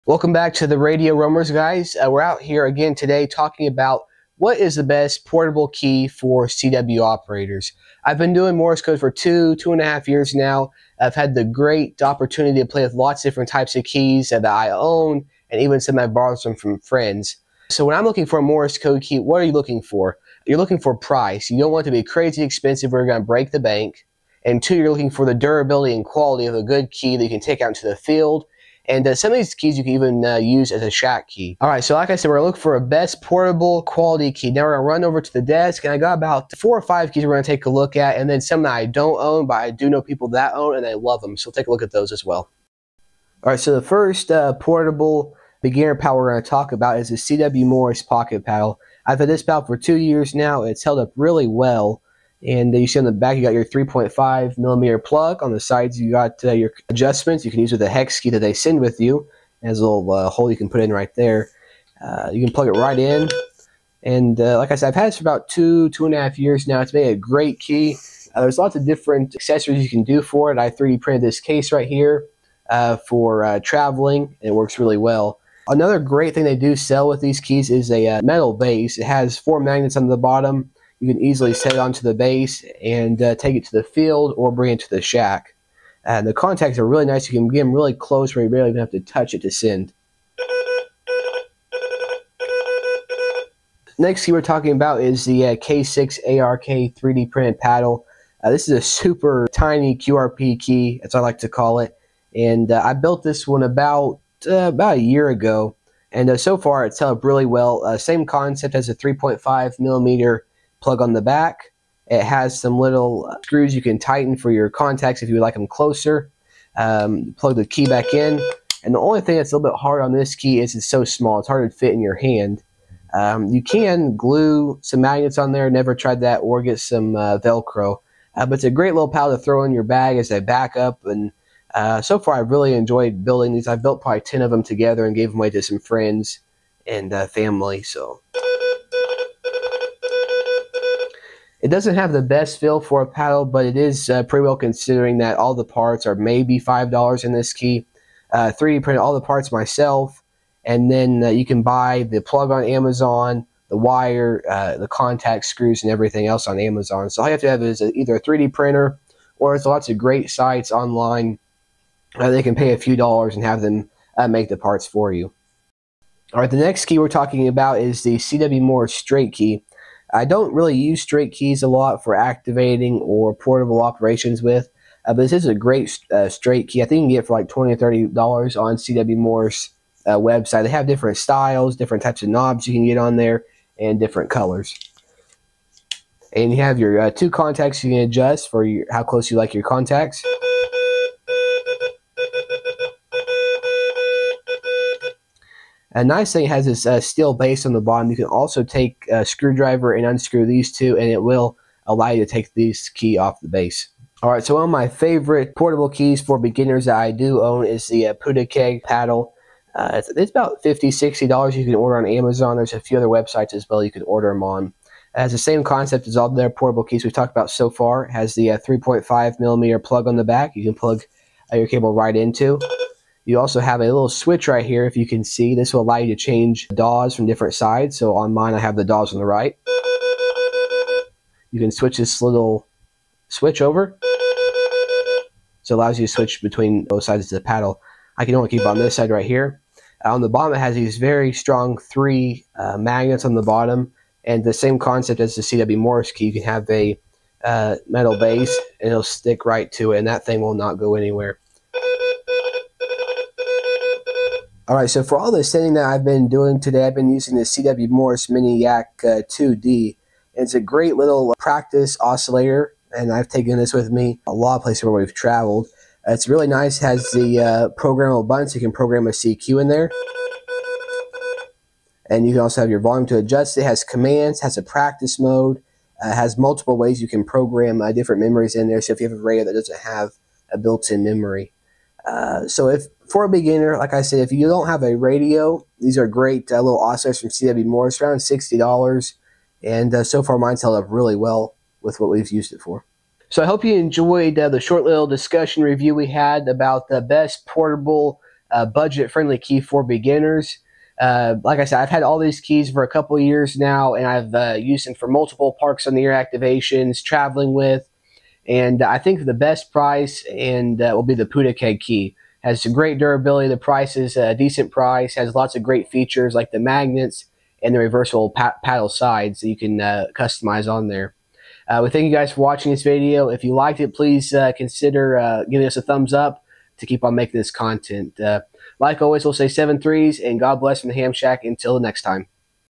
What is the best budget friendly portable Morse code key for beginners
Josiah gives us a brief description of best budget friendly portable Morse code keys.